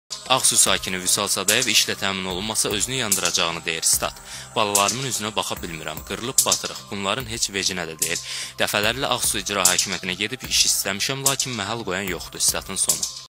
Vüsal Sadayev söyləyir ki, balalarımın üzünə baxa bilmirəm
Rayon sakini Vüsal Sadayev söyləyir ki, balalarımın üzünə baxa bilmirəm. Evin altında qırılıb-batırıq, bunların heç vecinə deyil.
"İcra başçısı Ənvər Seyidəliyevin özü ilə görüşdüm. Dedi ki, get, sabah gələrsən. Hər dəfə müxtəlif bəhanələr gətirib, məni qəbul etmədi. Axırda da sözü bu oldu ki, mən sənə nə etməliyəm, get, iş tap, işlə də. İş o yerə çatdı ki, icra başçısına dedim ki, benzin töküb, özümü yandıracam. Bircə onda bir az əl-ayağa düşdü, dedi ki, hə, yaxşı, sənin işini düzüb-qoşacağıq. Sonra təkrar getdim ki, bəs nə oldu? İcra başçısı dedi ki, get, əlindən nə gəlirsə, et"
Şikayətçi deyir ki, iş üçün dəfələrlə Ağsu Rayon Məşğulluq Xidmətinə də baş vurub. Lakin heç bir nəticəsi olmadığı üçün artıq ümidini hər yerdən üzüb: Məşğulluqda 250 manat məvacibi olan işləri imkanı olan ailələrin uşaqlarına təklif edirlər. Çünki əvəzində rüşvət alırlar. Məndən nə alacaqlar? Pulum var? Heç nə, yalandan vəd verib, süründürürlər. Əmək və Əhalinin Sosial Müdafiəsi Nazirliyi
Qarşı tərəflərin cavabları:
Ağsu rayon İcra Hakimiyyətinin ictimai -siyasi və hümanitar məsələlər şöbəsindən (Kenan Tadjizade) bildirildi ki, Vüsal Sadayev ad-soyadlı şəxsin məktubu onlara daxil olmayıb. Əlavə etdilər ki, bəzən vətəndaşlar özləri məsuliyyətsizlik edir.
Yaxud da iş üçün gəlib, təklif olunan işi istəməyib. Əsasən, təklif olunan işlər fəhləlikdir. Diplomu, təhsili olanları da təyinatına uyğun işlə təmin edirik. Olmayanlara da fəhlə işi təklif edirik, istəmirlər. Deyirlər ki, oturaq evimizdə, əlimizi ağdan-qaraya vurmayaq, pulumuz cibimizə gəlsin. Bizdə adətən, hansısa bir tikintidə, abadlıq işində fəhlə qüvvəsi tələb olunur, müvəqqəti işlər təklif edilir. Bəzən daimi işlər də olur, amma nadir hallarda. İş istəyənlər qeydiyyatdan keçməlidir. Elə adam var, gəlir, iş istəyir, qeydiyyatdan keçməyib. Deyirik ki, hələ iş yoxdur, üzünü çevirib, çıxıb gedir. Elə vətəndaşlara biz nə edə bilərik? Biz o adamların arxasınca düşüb, axtaran deyilik. Bir il əvvəl iş istəyib, sonra gəlib, maraqlanmayıb. Bu nə dərəcədə doğrudur? İldə bir dəfə iş üçün gəlir. Deyirik ki, indi iş yoxdur, olanda verərik, çıxıb gedir. Sonra da deyir ki, mənə iş vermədilər"
Rayon Məşğulluq Xidmətindən bildirilib ki, Vüsal Sadayev təkrar bizə müraciət etsin, onun işlə bağlı şikayətinə baxılacaq:
"O, iki il əvvəl işlə bağlı müraciət edib. Bu müddət ərzində ona uyğun iş olmayıb. Yaxınlaşıb, sənədlərini yeniləsin".
Vüsal Sadayev isə hər iki qurumun mövqeyi ilə razı deyil: "Yalan danışırlar. Dəfələrlə iş üçün getmişəm. Məni görməzlikdən gəlirlər. İcrada iş vermək istəmirlər. Məşğulluq Xidmətinə dünən - dekabrın 18-də yaxınlaşdım. İş üçün sənədlərimi də təkrar yenilədim. Görüm, bundan sonra bəhanələri nə olacaq".
(Mənbə: MeydanTv)